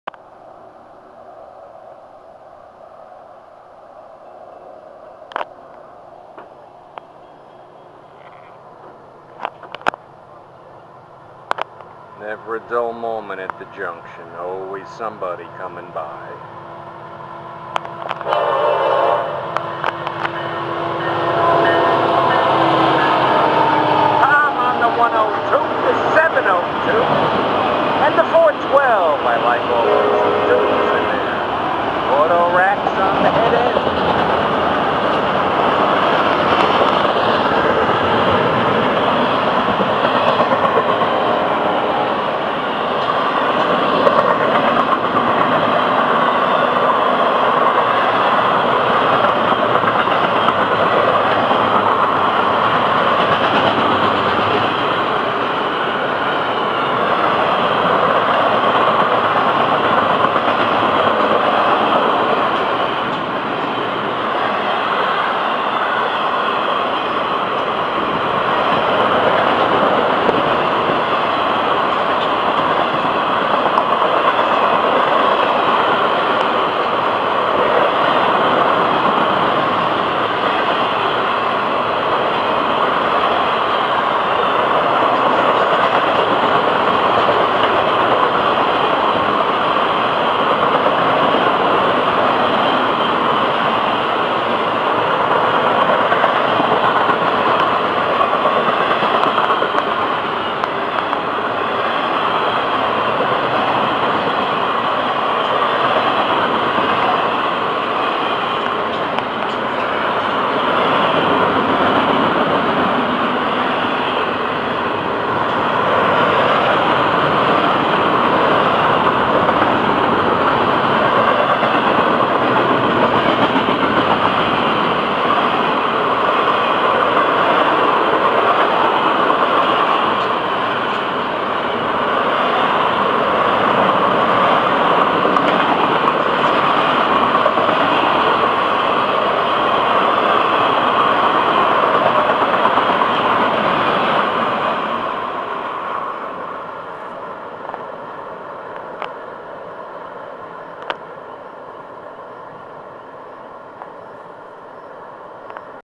FEC101 Jun 4, 2013
Tom waving to Ryan Lee with both hands from 102 and 702, 412 leading racks and IM.
cabooseMikey, Ryan Lee and Fred, and Brylon, too.